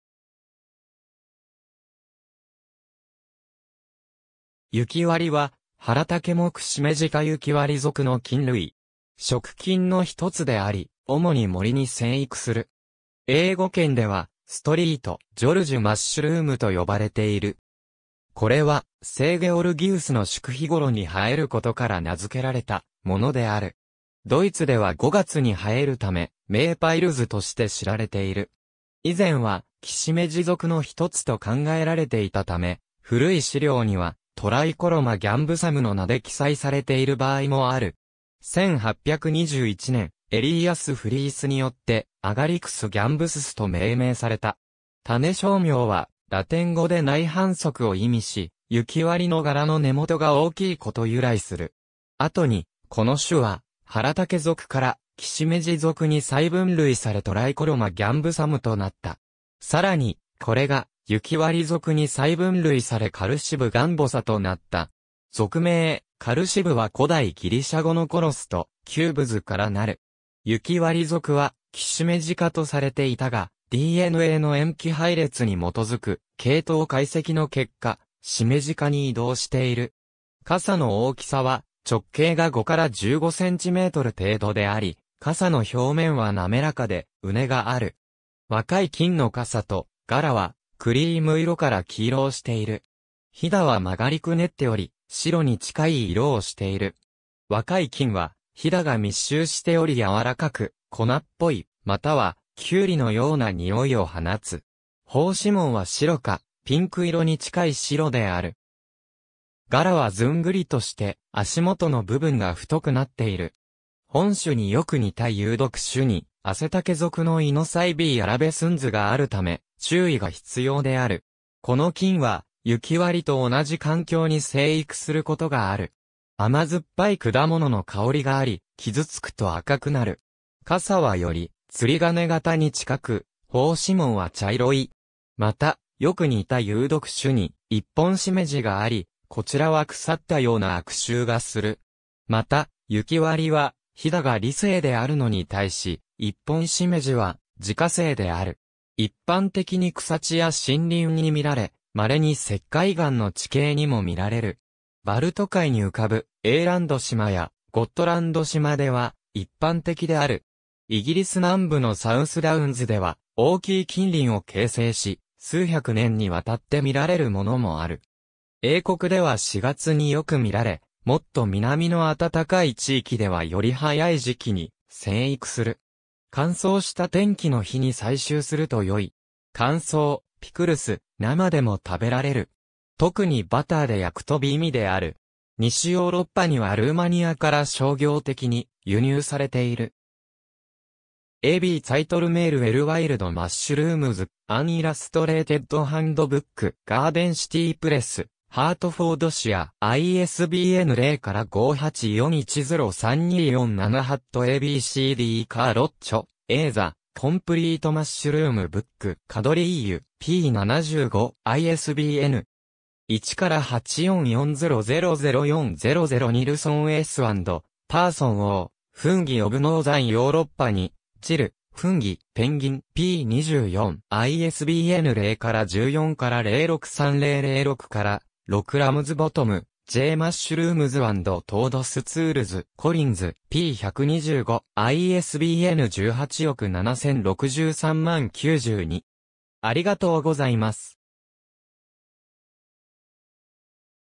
ユキワリ
ユキワリ（雪割：学名：Calocybe gambosa）は、ハラタケ目シメジ科ユキワリ属の菌類。
食菌の一つであり、主に森に生育する。
英語圏ではSt. Georges mushroom（聖ゲオルギウスのきのこ）と呼ばれている。
これは聖ゲオルギウスの祝日（4月23日）ごろに生えることから名づけられたものである。
ドイツでは5月に生えるため、Maipilz（五月キノコ）として知られている。
以前はキシメジ属の一つと考えられていたため、古い資料にはTricholoma gambosumの名で記載されている場合もある。
1821年、エリーアス・フリースによってAgaricus gambosusと命名された。
種小名はラテン語で内反足を意味し、ユキワリの柄の根元が大きいこと由来する。
後にこの種はハラタケ属からキシメジ属に再分類されTricholoma gambosumとなった。
更にこれがユキワリ属に再分類されCalocybe gambosaとなった。
属名Calocybeは古代ギリシャ語のkalos（美しい）とcubos（頭）から成る。
ユキワリ属はキシメジ科とされていたが、DNAの塩基配列に基づく系統解析の結果シメジ科に移動している。
傘の大きさは直径が5～15cm程度であり、傘の表面はなめらかで、うねがある。
若い菌の傘と柄はクリーム色から黄色をしている。
襞は曲がりくねっており、白に近い色をしている。
若い菌は襞が密集しており柔らかく、粉っぽい、またはキュウリのようなにおいを放つ。
胞子紋は白か、ピンク色に近い白である。
柄はずんぐりとして、足元の部分が太くなっている。
本種に良く似た有毒種に、アセタケ属のInocybe erubescensがあるため、注意が必要である。
この菌はユキワリと同じ環境に生育することがある。
甘酸っぱい果物の香りがあり、傷つくと赤くなる。
傘はより釣鐘型に近く、胞子紋は茶色い。
また、よく似た有毒種にイッポンシメジ（Entoloma sinuatum）があり、こちらは腐ったような悪臭がする。
また、ユキワリは襞が離生であるのに対し、イッポンシメジは直生である。
一般的に草地や森林に見られ、稀に石灰岩の地形にも見られる。
バルト海に浮かぶエーランド島やゴットランド島では一般的である。
イギリス南部のサウスダウンズでは大きい菌輪を形成し、数百年にわたって見られるものもある。
英国では四月に良く見られ、もっと南の暖かい地域ではより早い時期に生育する。
乾燥した天気の日に採集するとよい。
乾燥、ピクルス、生でも食べられる。
特にバターで焼くと美味である。
西ヨーロッパにはルーマニアから商業的に輸入されている。
^ a b Zeitlmayr L (1976). Wild Mushrooms:An Illustrated Handbook. Garden City Press, Hertfordshire. ISBN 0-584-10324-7 ^ a b c d Carluccio A (2003). The Complete Mushroom Book. Quadrille. p. 75. ISBN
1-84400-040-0 ^ Nilson S & Persson O (1977). Fungi of Northern Europe 2: Gill-Fungi. Penguin. p. 24. ISBN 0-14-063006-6 ^ Ramsbottom J (1953). Mushrooms & Toadstools. Collins. p. 125. ISBN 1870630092